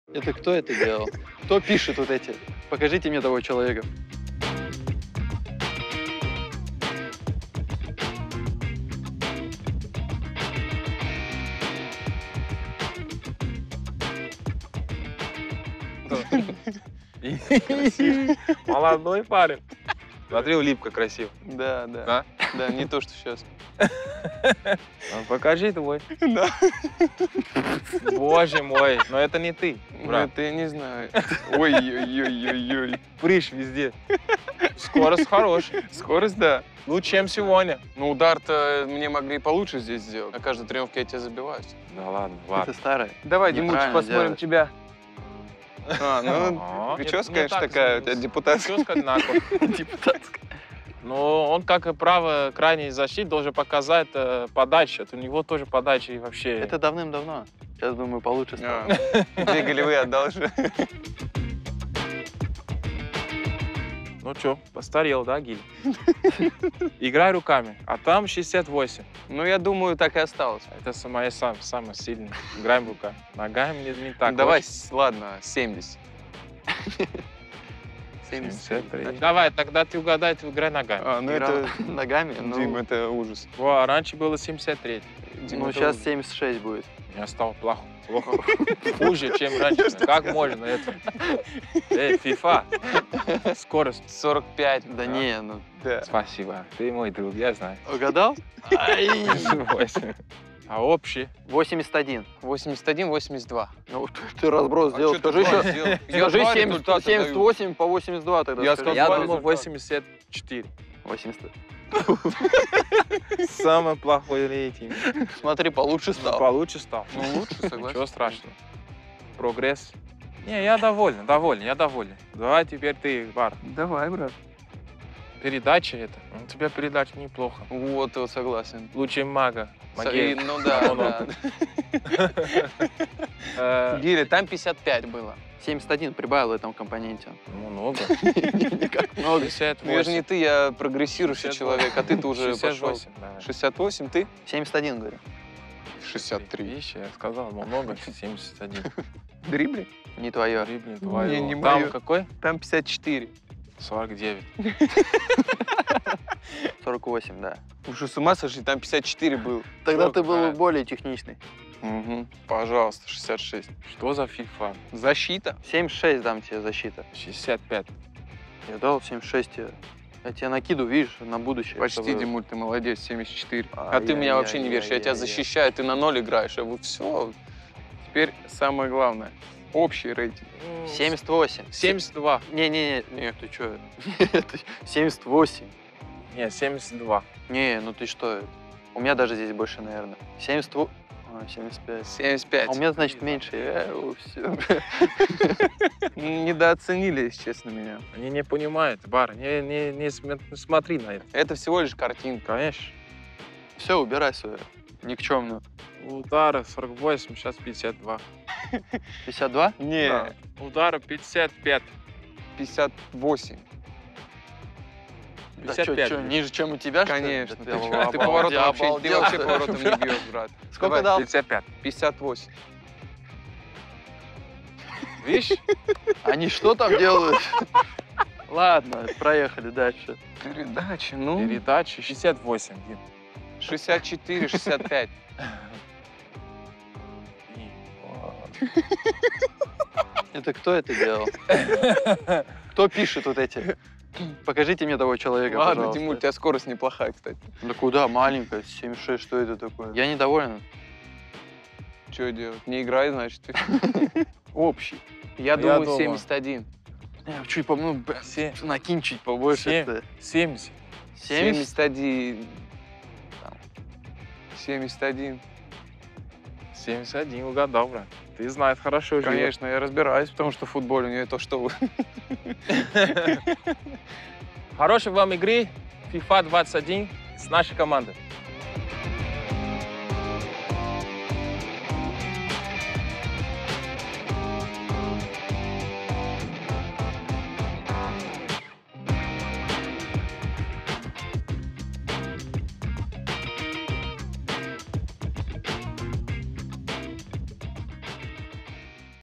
FIFA 21 // Гилерме, Баринов и Живоглядов узнают свои рейтинги
«Кто пишет вот эти рейтинги?! Покажите мне этого человека»
Гилерме, Баринов и Живоглядов очень весело сравнивают свои старые и обновлённые рейтинги в FIFA.
Ловите порцию хорошего настроения.